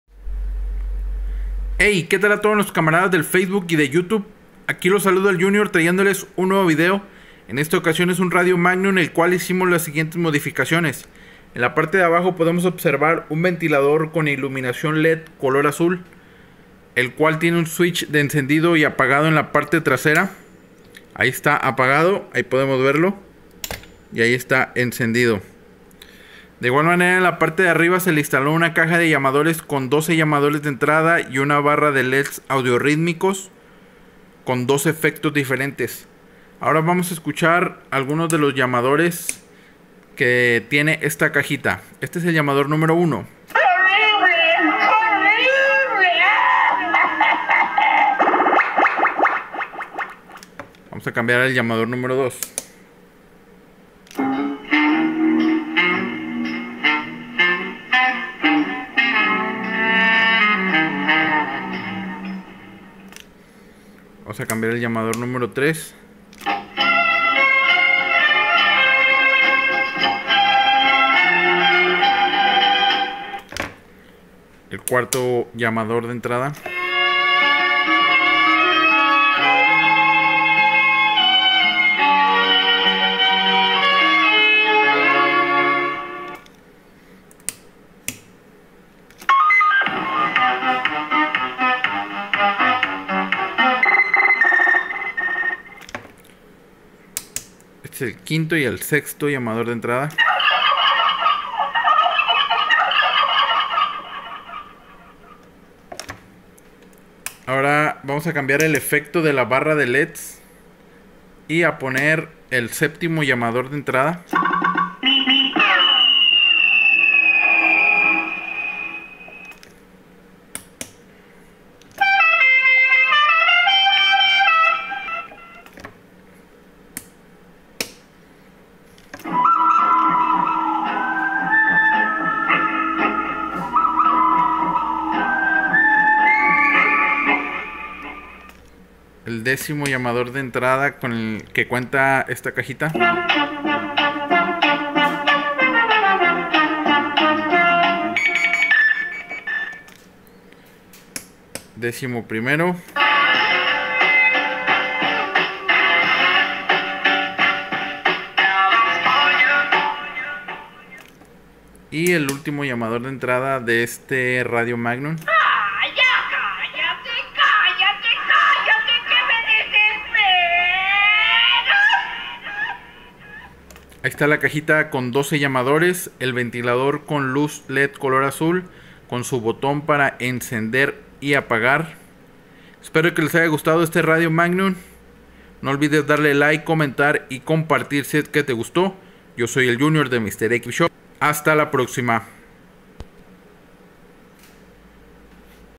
MODAS Y ESTILOS CB CUSTOM MAGNUM
Radio magnum con caja de llamadores con 12 entradas, una tira de Led's audioritmicos y Ventilador con iluminación Led azul